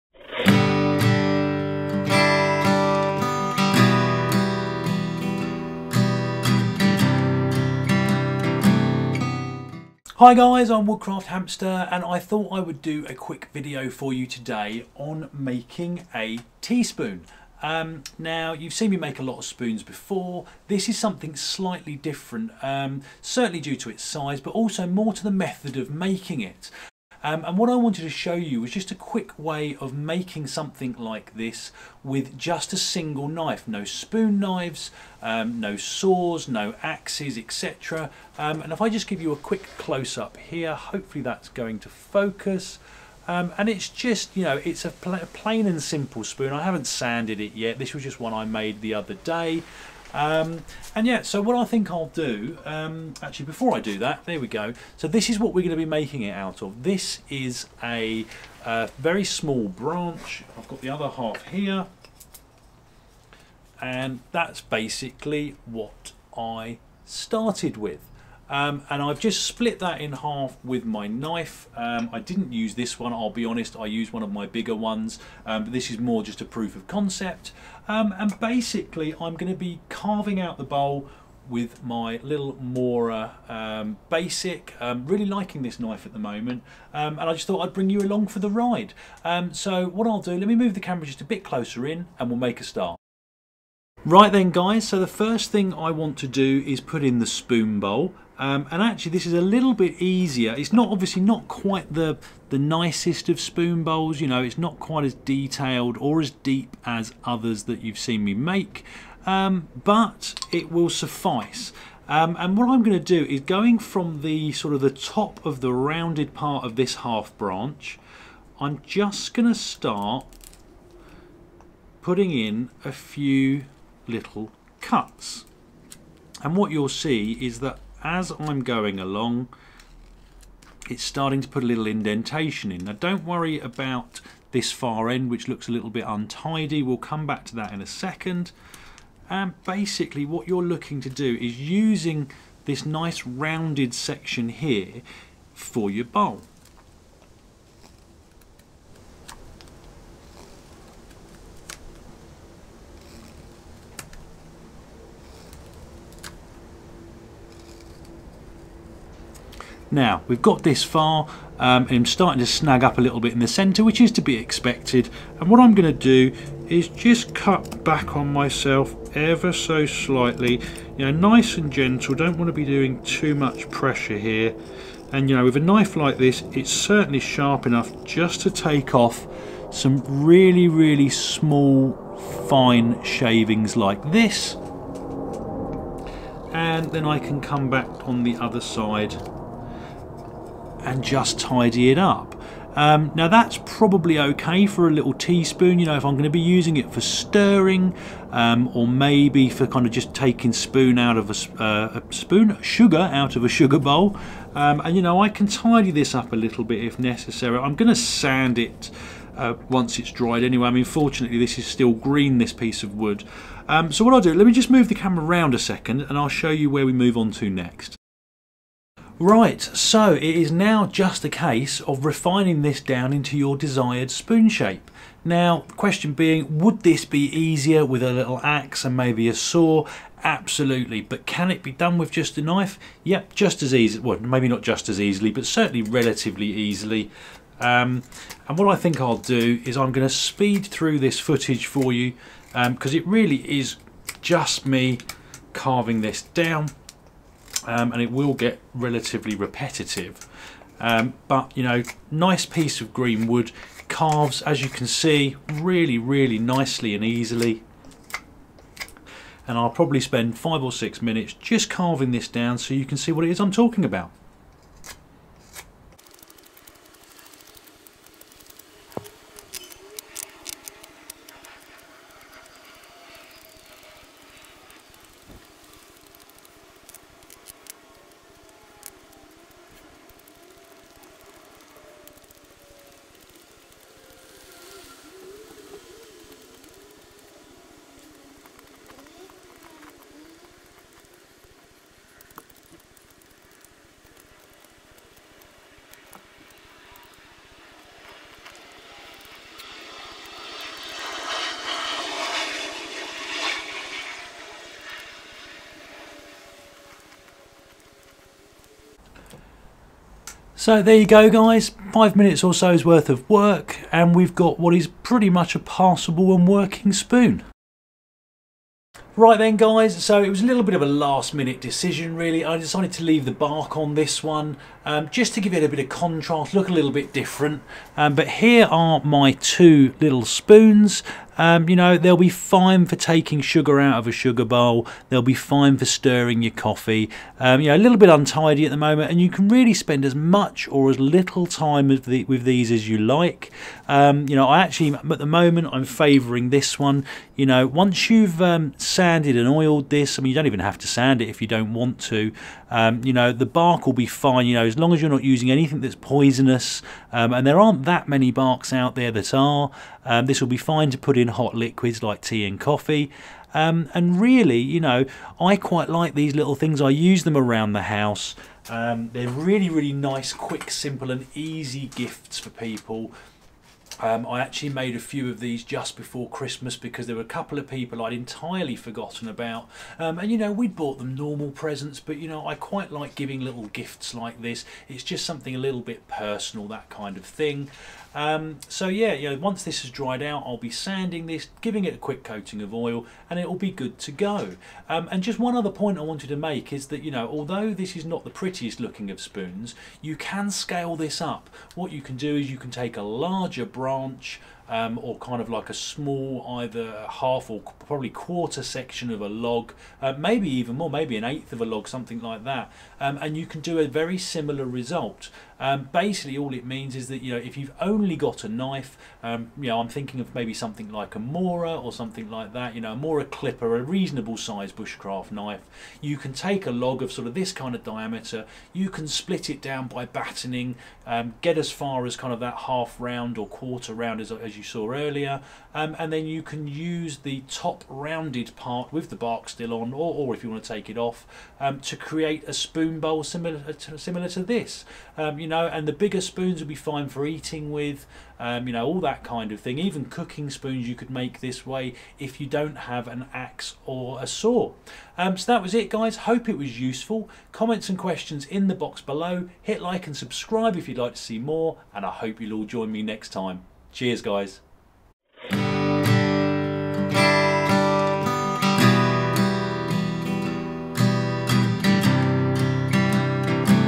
One Tool Carving Teaspoon With Just a Knife
A quick method of making a teaspoon with just your knife.

Great for the i am in the woodland want a slightly more elaborate stirrer than my usual twig.